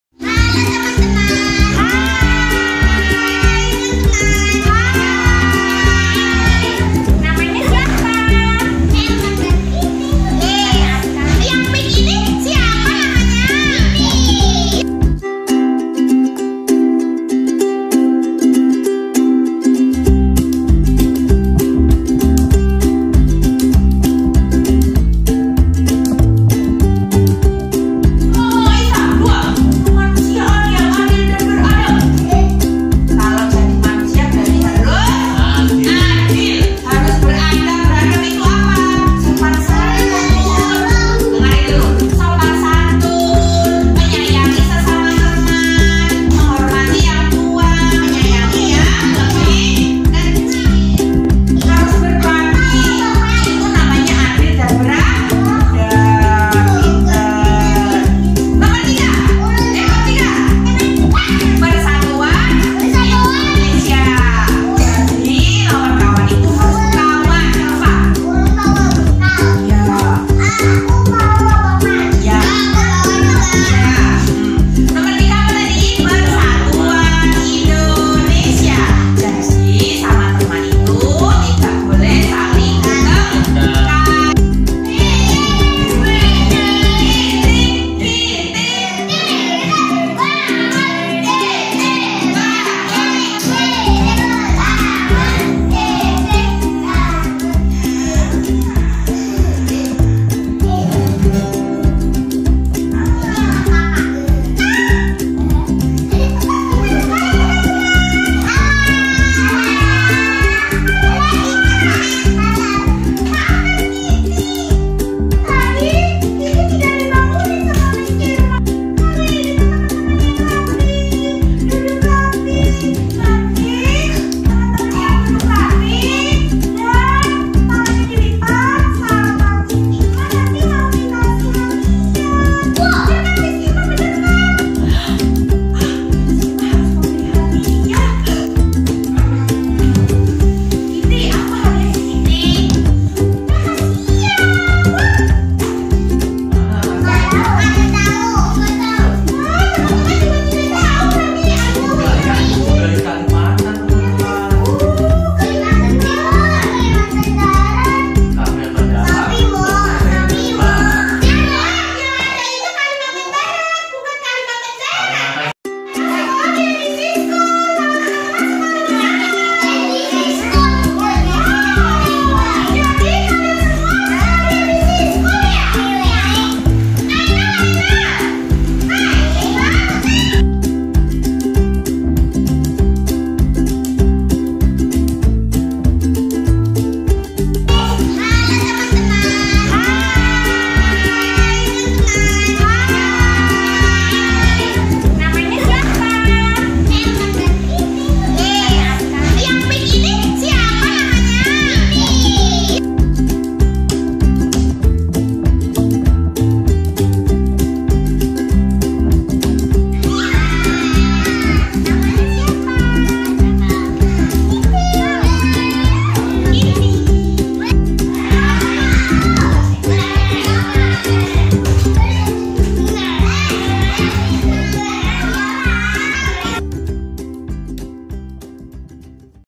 latihan berani tampil | sekolah si @Kang Nadil
kegiatan belajar disekolah si bungsu @kang Nadil hari ini,Senin 31 Januari 2022